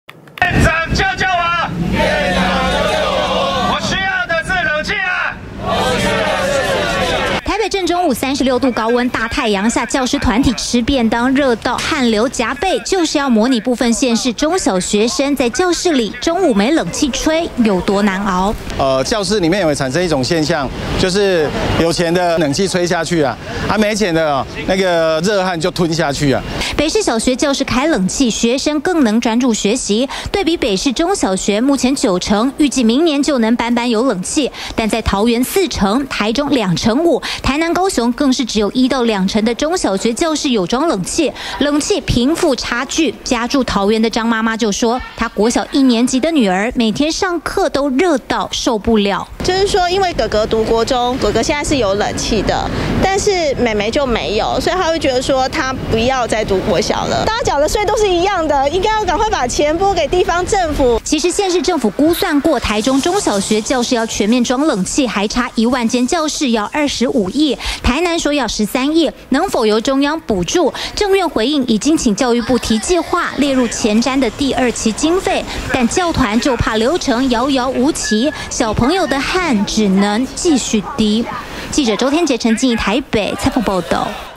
20200623中天新聞 教室裝冷氣南部僅1成！ 教團轟「有錢縣市爽吹」
連日高溫，民眾開冷氣消暑，但是中小學生上課，卻不是人人都有冷氣吹。像是台北市，明年就能班班有冷氣，但是南部縣市的學生，卻得忍受高溫，明顯不公平。教師團體出面喊話，希望中央編列預算，補助各縣市中小學裝冷氣。月底前沒得到答案，還會有更大規模的抗議行動。